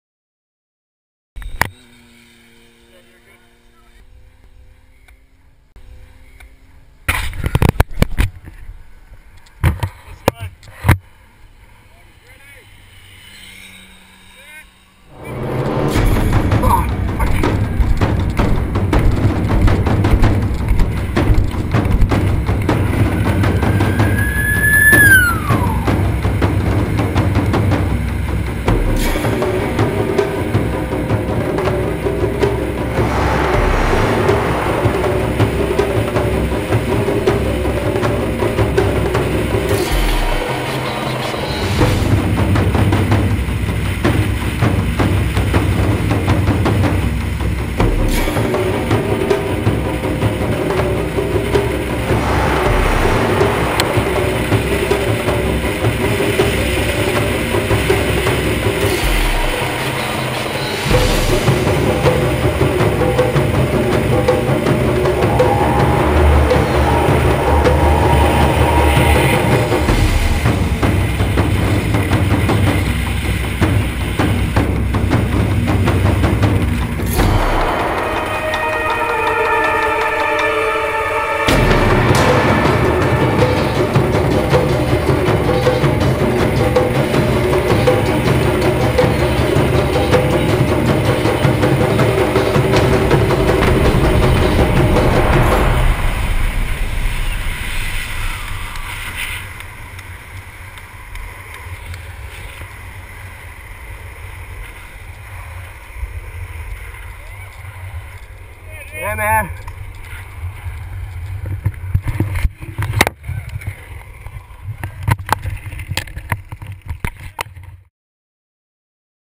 RADELAIDE OUTLAW FINAL
The final race of the day. Was a bit of a rush as we thought the siren coming up the hill was a cop, but was still a good race and finished the day well.